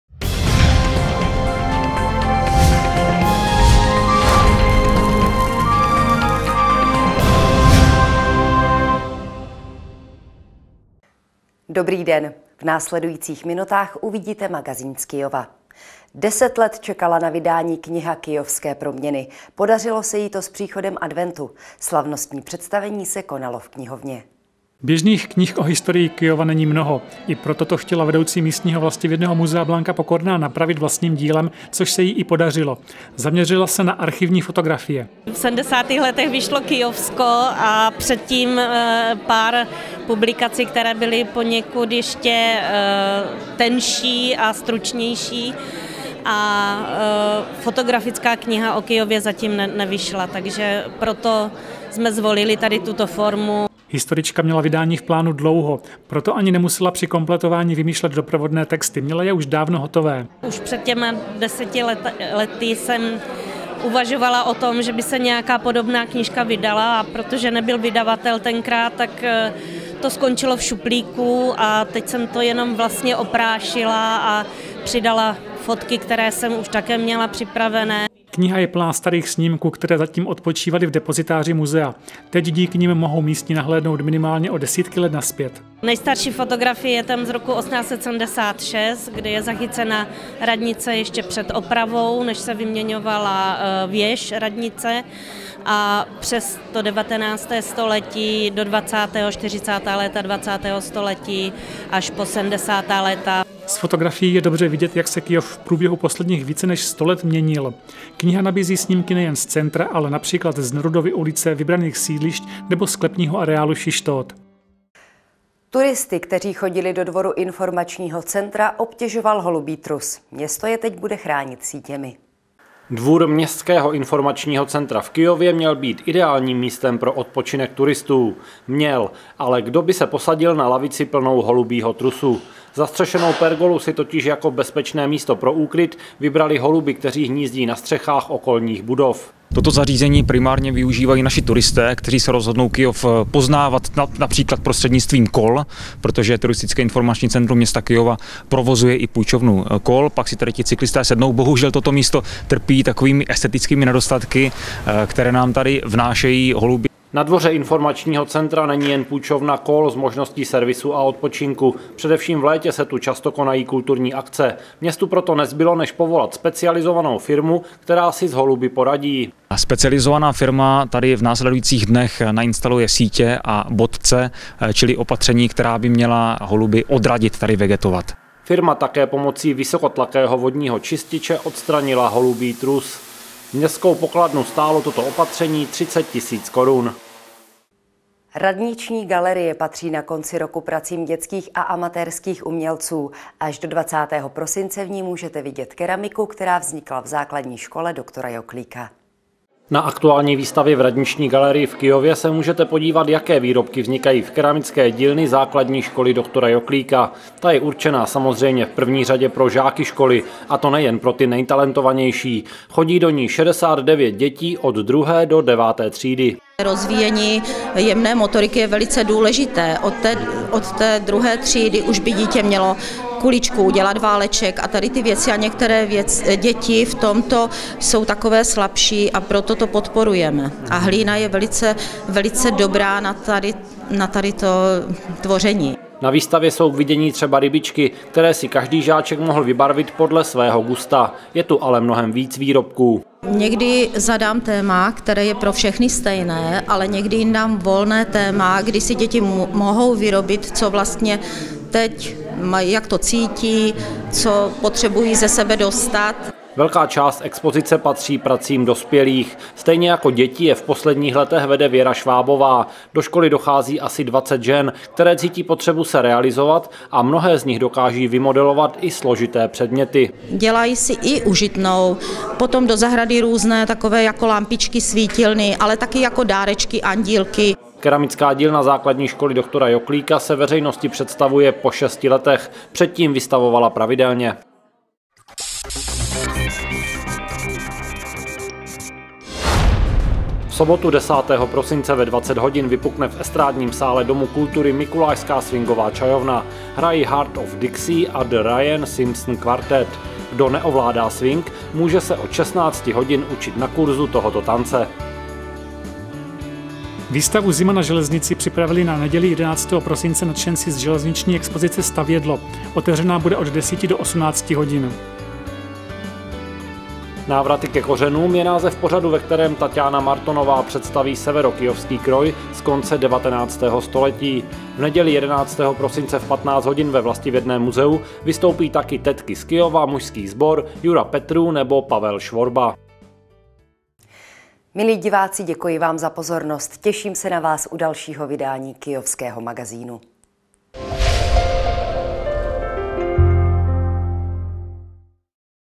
TVS: Kyjov 6. 12. 2016
Vyšla fotokniha o proměnách Kyjova / V Ičku bojují proti holubům sítěmi / Keramický kroužek ZŠ Dr. Joklíka se představi lna výstavě v Radniční galerii